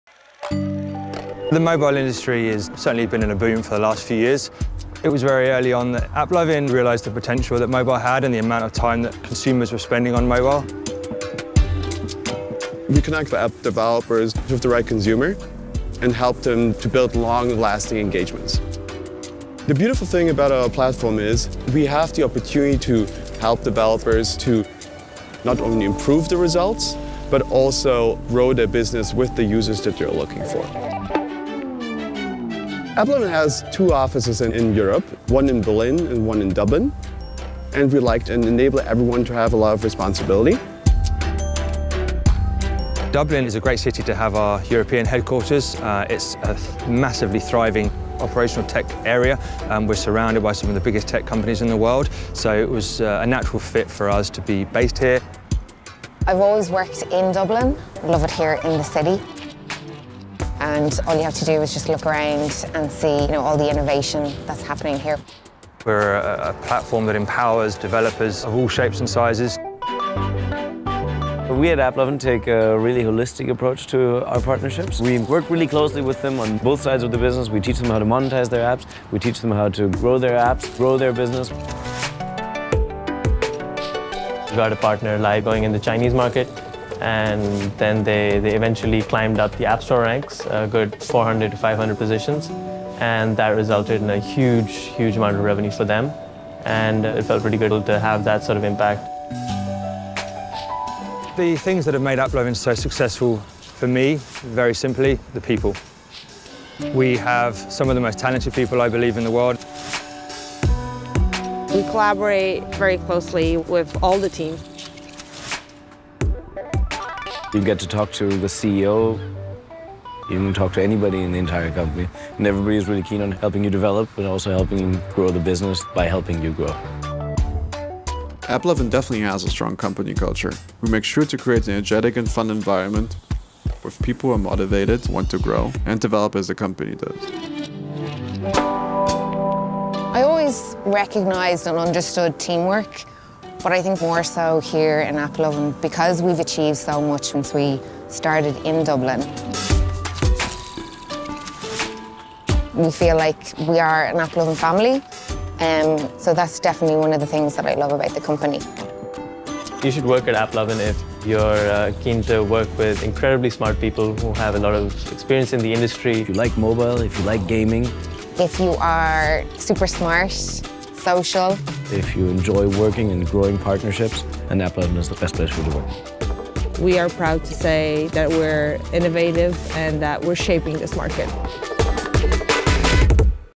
Applovin // Dublin
Applovin is a tech company that connects app developers with the right consumers to build long-lasting engagements. Its teams help empower developers all over the world to improve results. With offices in the US, China, Berlin, and its European headquarters in Dublin, Ireland, Applovin creates a strong, positive work culture in each of its regions, priding itself on empowering employees to build healthy (and fun) lives for themselves and their communities.

Directed and produced by: Nicole Sorochan - One Net Inc.
Director of photography: Jan Vozenilek - Copper Sky Productions